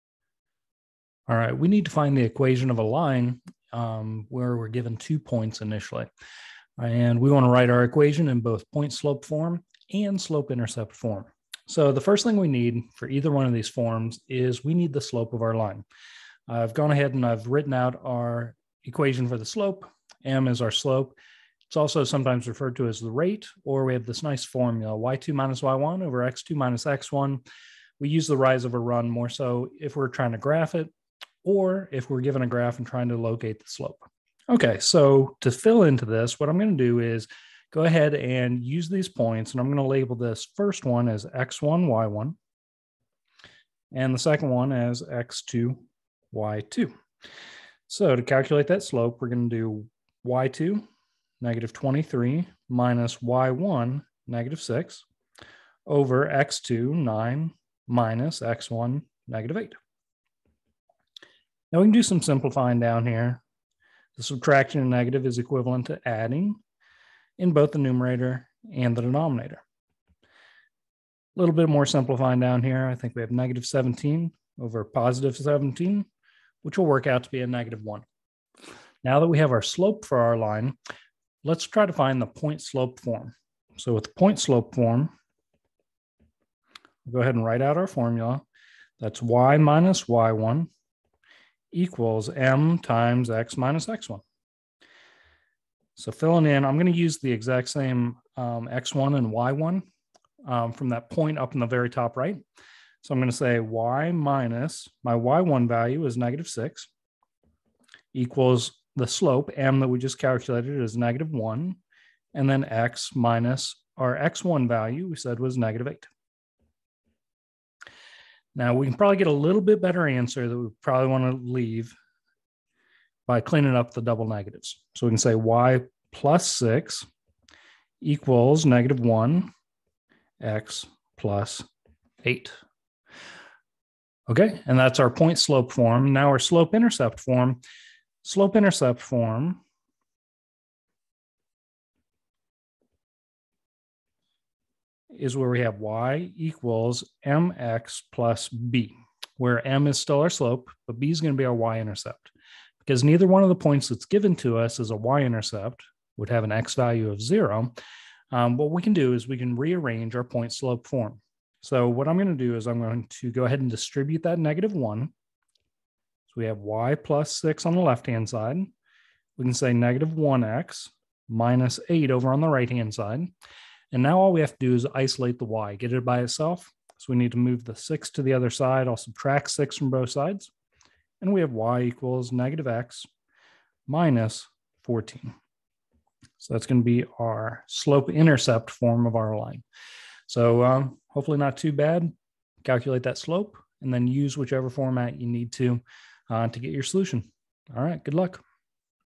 Find linear equation (line) given two points in point-slope form and slope-intercept form
In this example problem, we are given two points on a line and find the slope of the line using the slope formula, then the equation of the line in both point-slope form and slope-intercept form.